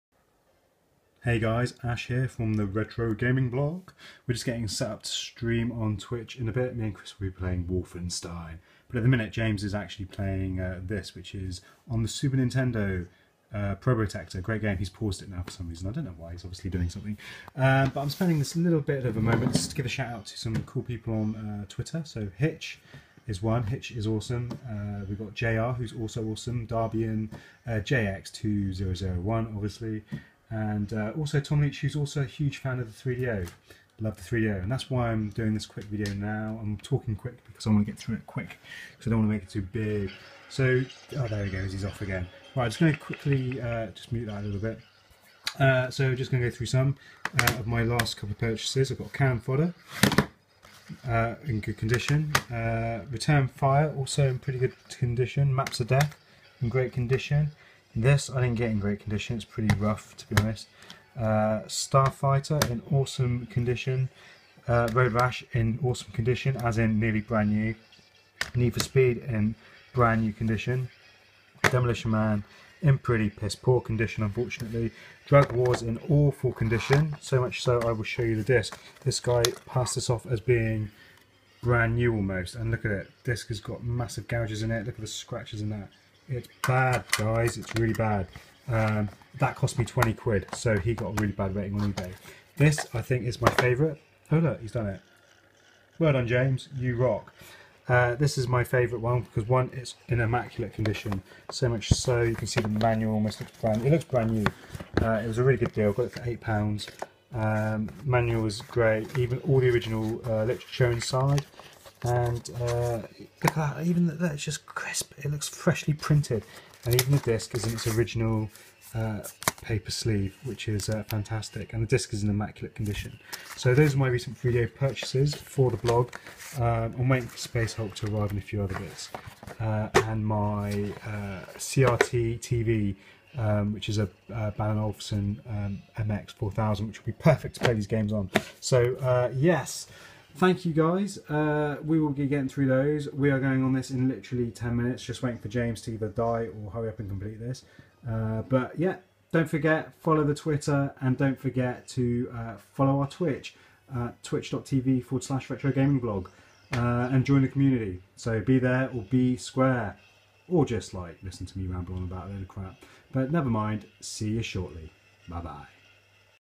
Latest 3DO Retro Pickups - RGB
RetroGamingBlog's latest Retro pick ups. Shout out to Hitch, JX2001, JR_UK, RETROJOE, Tom leech and more. You guys all rock!